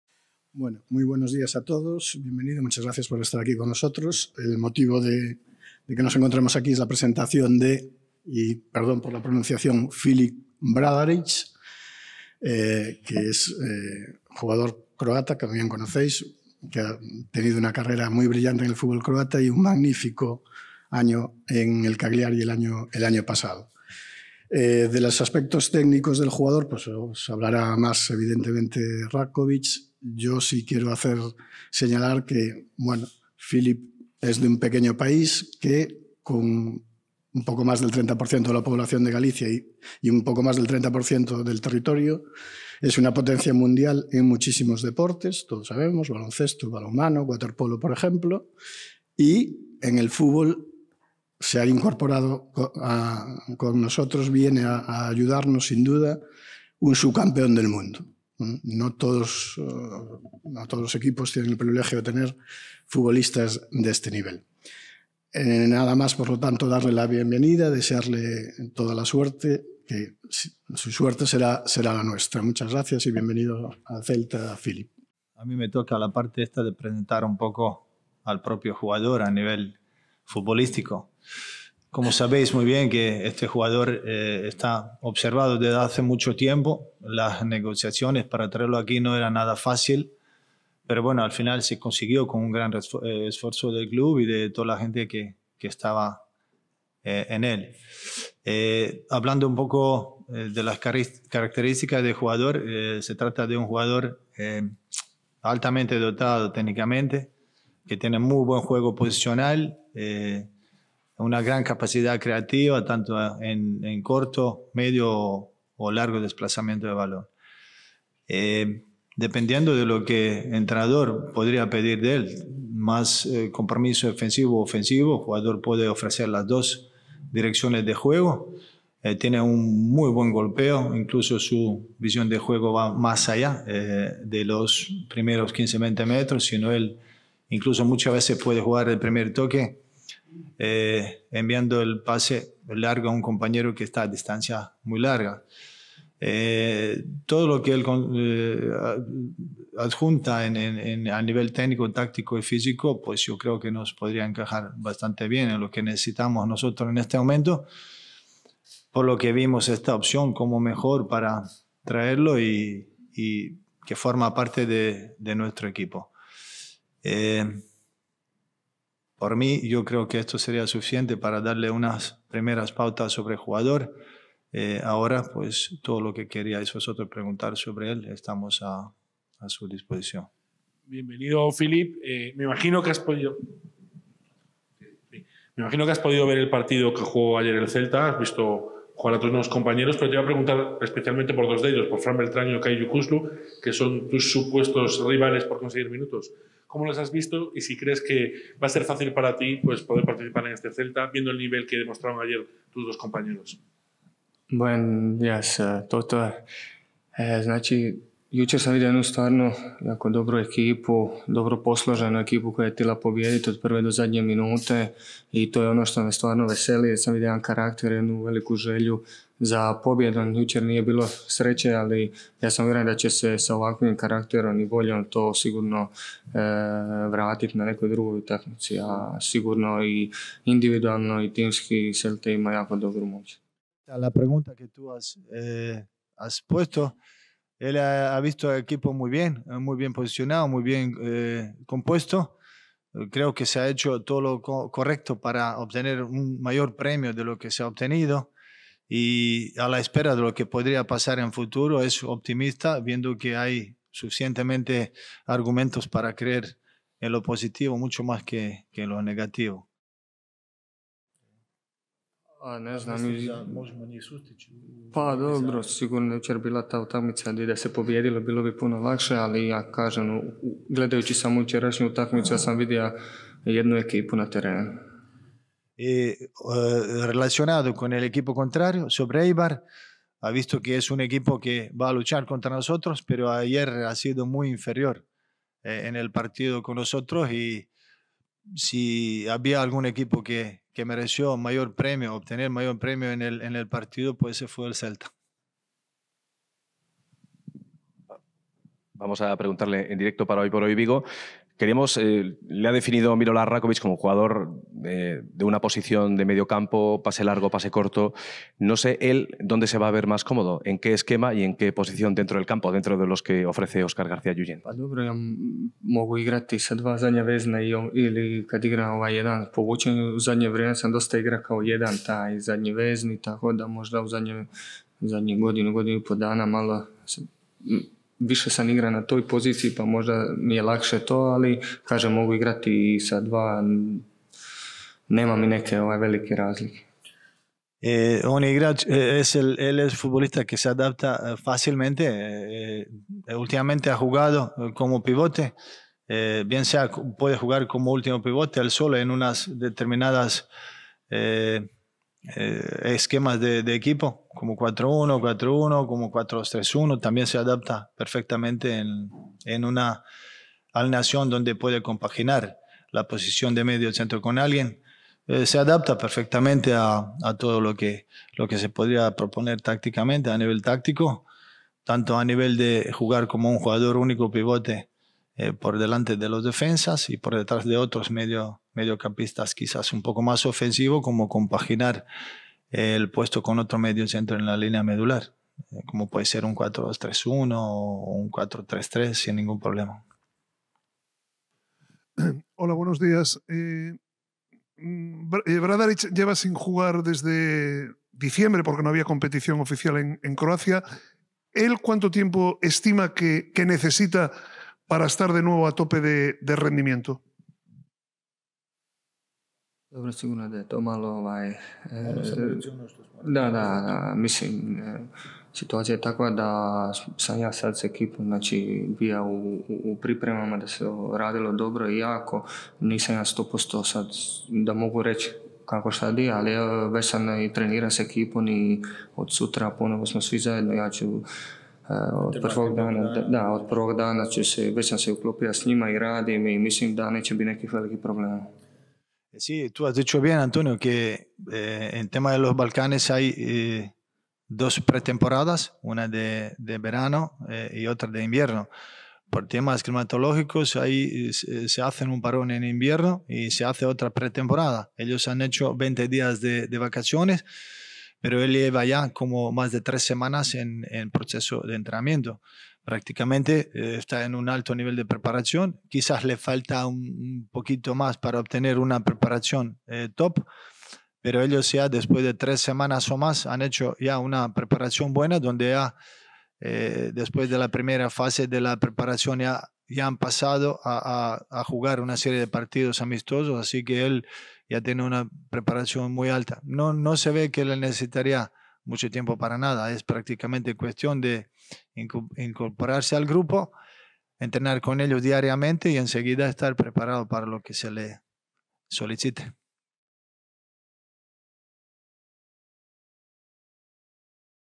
Presentación oficial de Filip Bradarić
Filip Bradarić, nuevo centrocampista celeste y segundo fichaje invernal del RC Celta tras la incorporación de Jeison Murillo, se presentó oficialmente ante los medios de comunicación en el Salón Regio de A Sede. El internacional croata, que formó parte de la convocatoria de Óscar García ante el Eibar, estuvo acompañado en su puesta de largo como futbolista céltico por el consejero Primitivo Ferrero y el miembro de la secretaría técnica y ex jugador celeste Milorad Ratkovic.